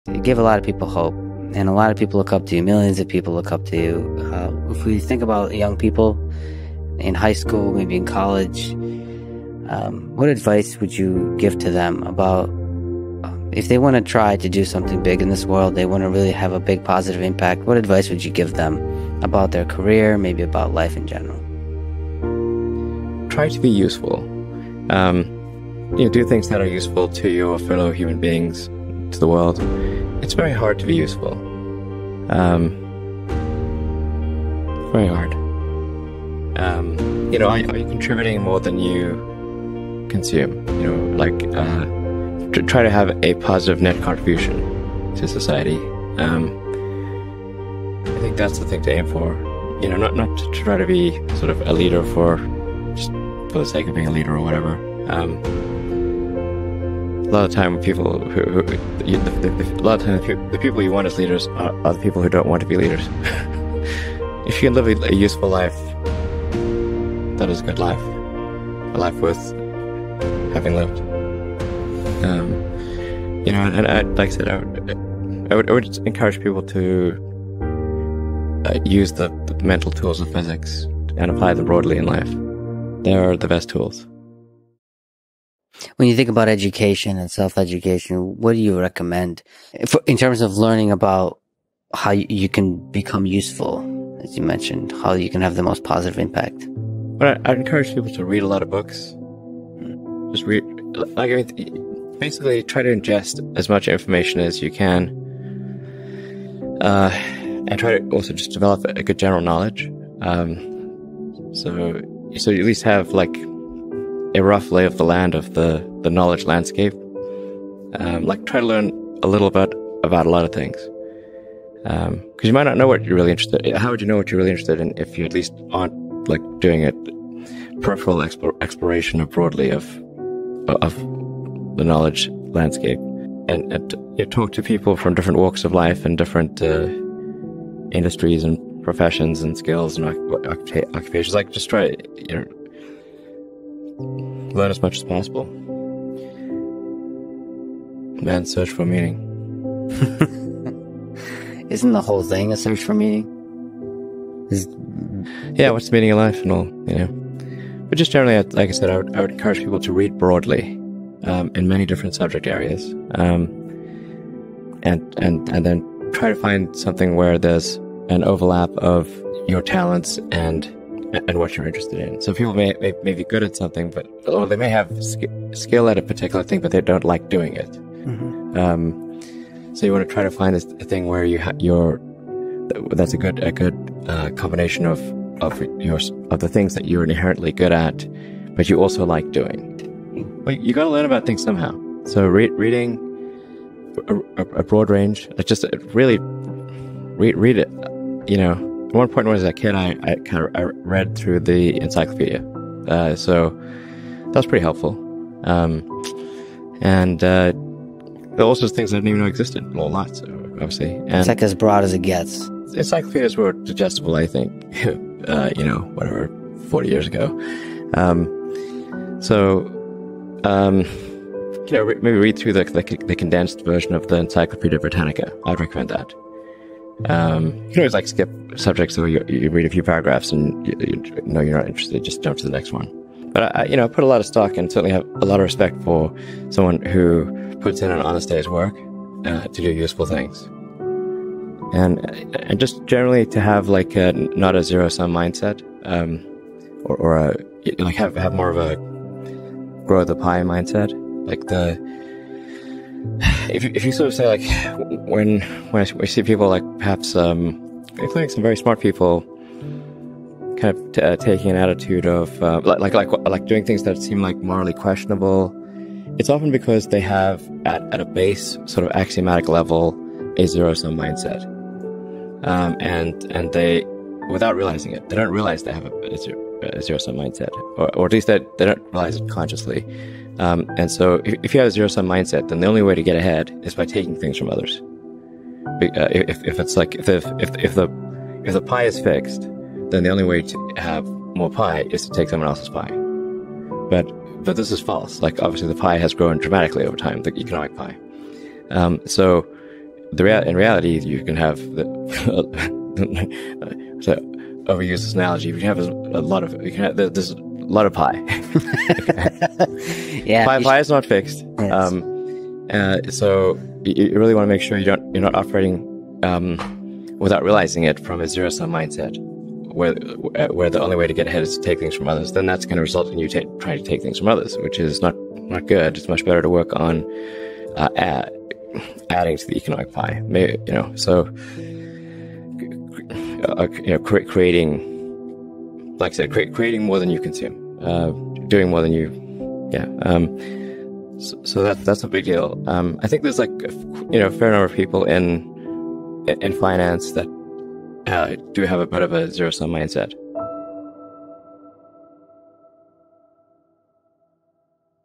Elon Musk's Radical Advice: BE USEFUL!!!
Discover the life philosophy of one of the most brilliant and controversial figures of our time, Elon Musk. In this video, we delve into his famous quote, "be useful", and explore how it drives his decisions, his businesses, and his vision for the future. From SpaceX to Tesla, from solar energy to brain-computer interfaces, Elon Musk is always looking for ways to make the world a better place through innovation and entrepreneurship. If you want to understand the mindset of a modern visionary and learn how to make a difference in your own life, watch this video and get inspired by Elon Musk's wisdom.